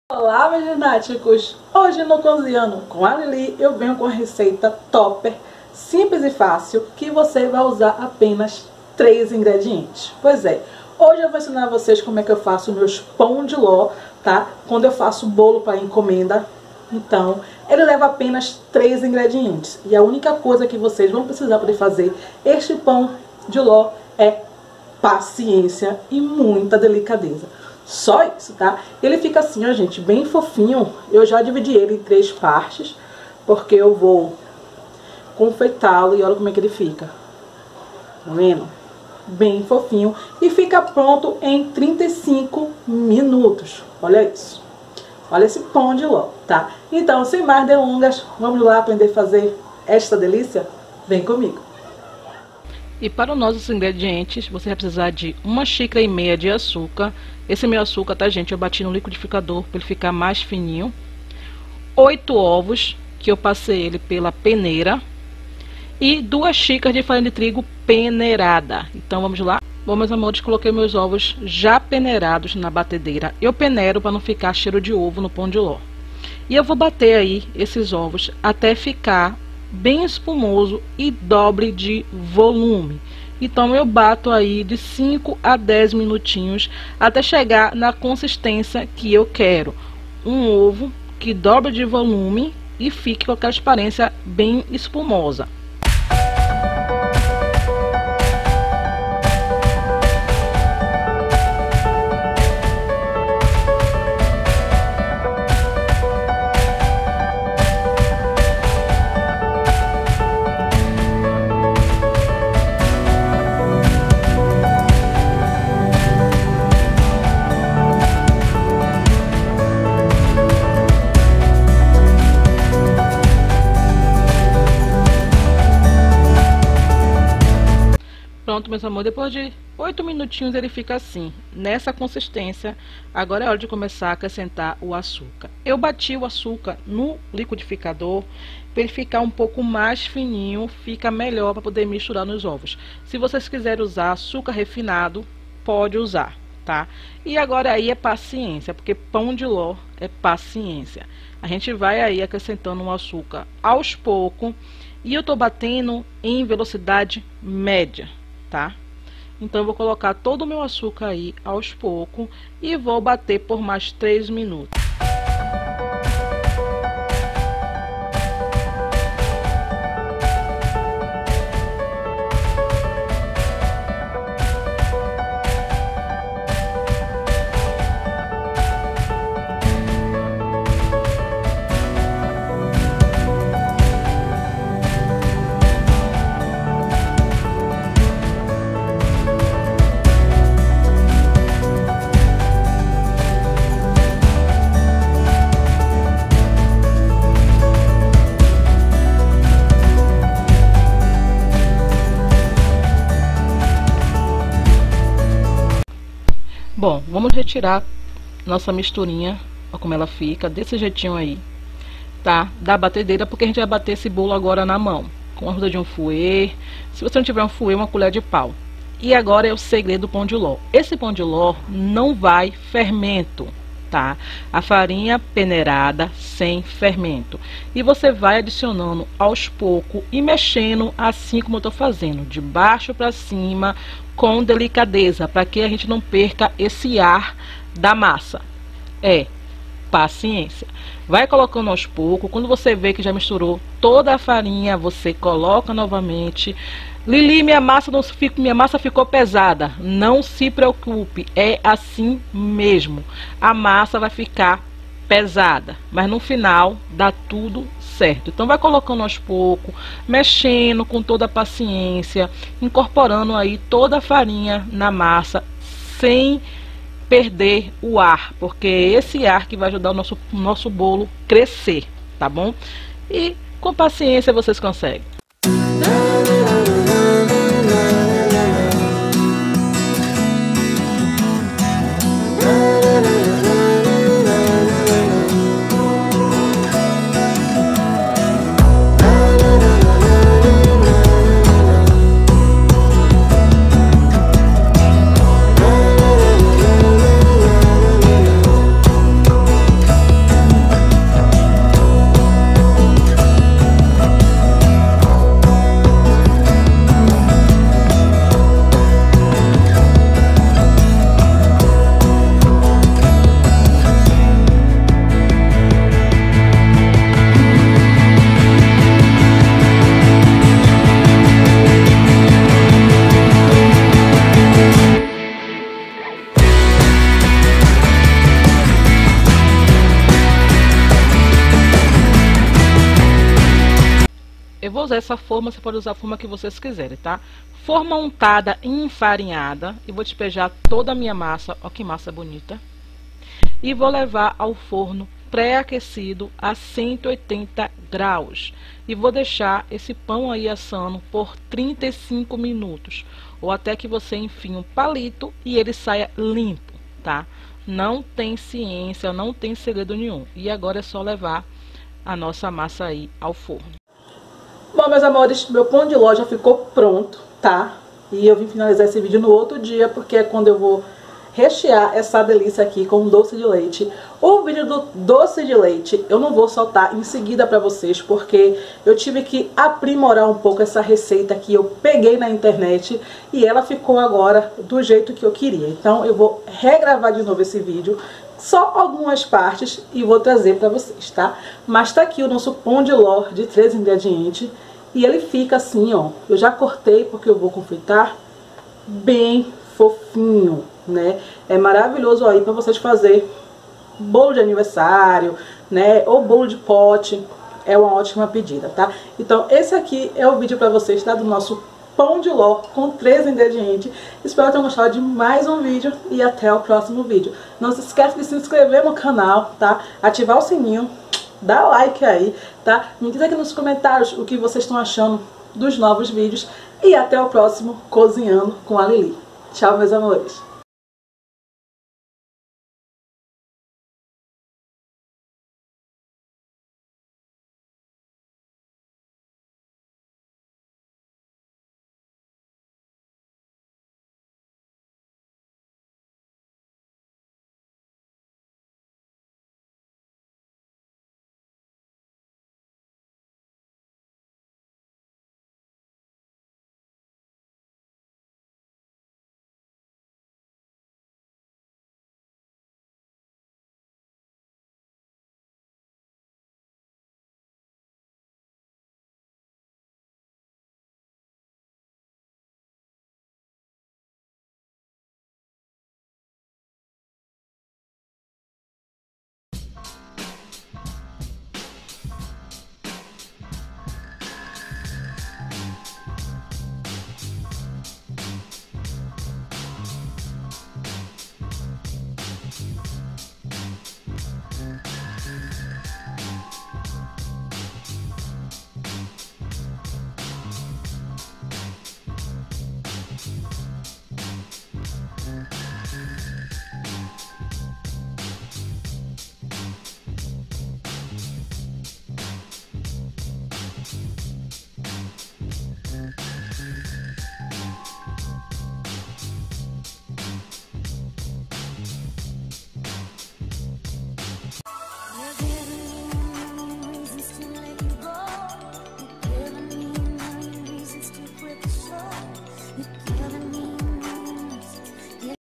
Pão de ló com 3 ingredientes
▬▬▬▬▬▬▬▬▬▬▬ஜ۩۞۩ஜ▬▬▬▬▬▬▬▬▬▬▬▬▬
Olá meus amores hj eu trago um pão de  ló simples com apenas 3 ingredientes, e dá pra fazer bolo de pote ou de aniversário.
Ingredientes
8 ovos peneirados
1 1/2 xícara de açúcar
2 xícara de farinha de trigo